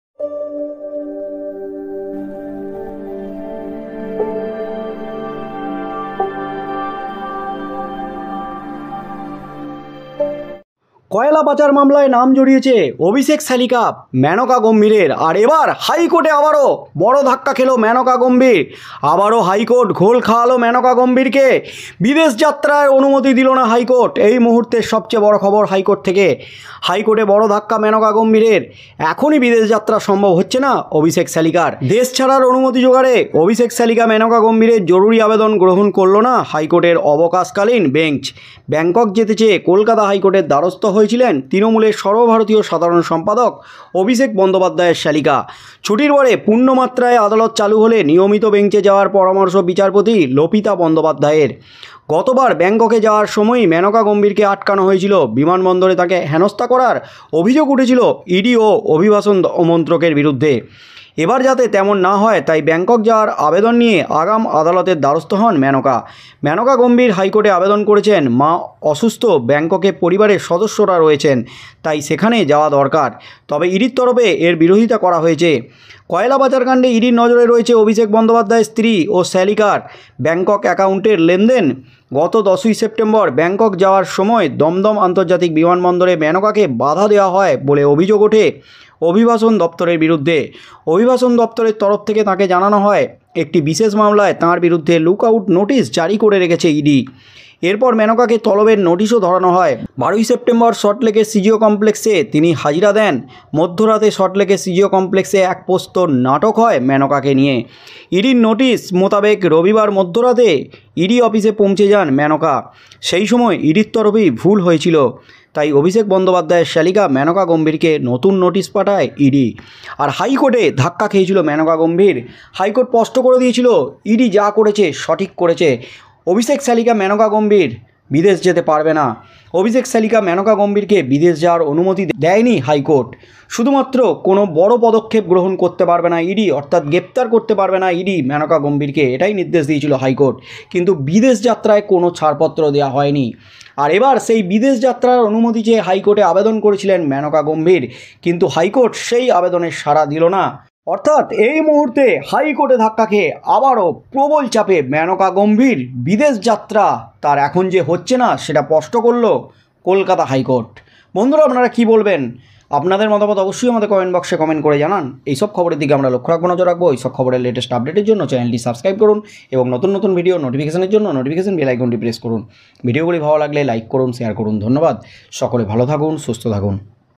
অভিষেক শ্যালিকা কে আবারও ঘোল খাওয়ালো হাইকোর্ট! বিচারপতির কড়া নির্দেশ! হাইকোর্টের ধাক্কায় বেশামাল
ধন্যবাদ

Keyword⏩